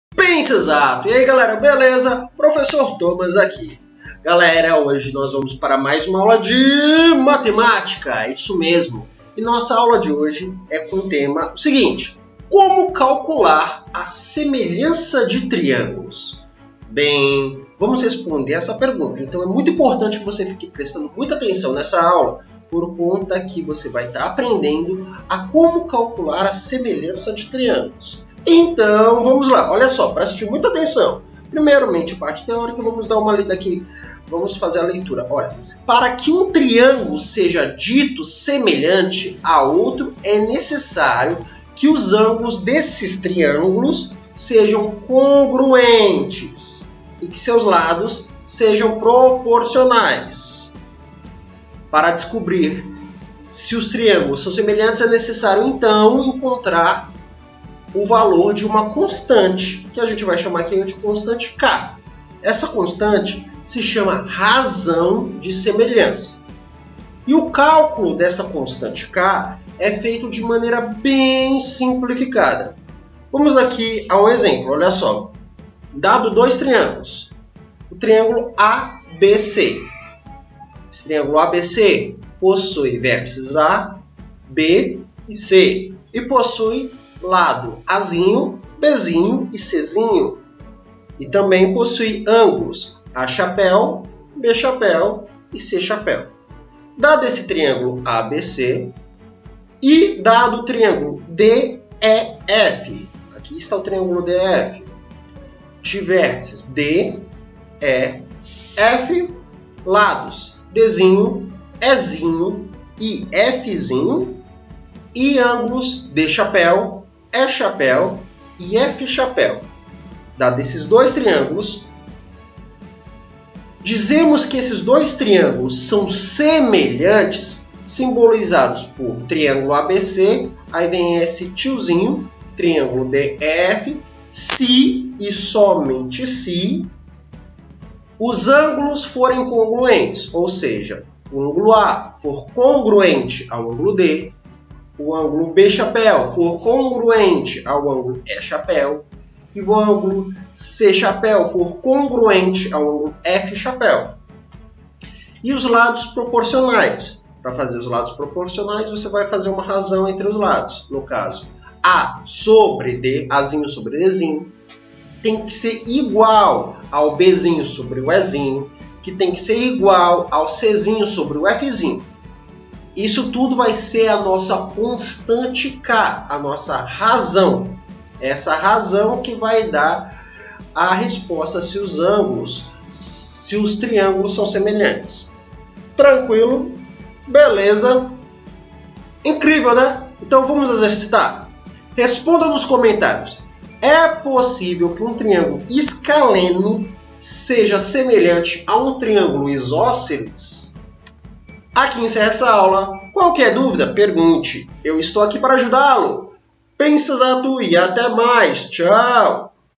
COMO CALCULAR A SEMELHANÇA DE TRIÂNGULOS? Matemática (EF09MA12)
Utilizando de todo conhecimento de matemática desvende todos os mistérios da semelhança de triângulos, aprendendo a calcular de modo bem fácil.
Habilidade da BNCC: EF09MA12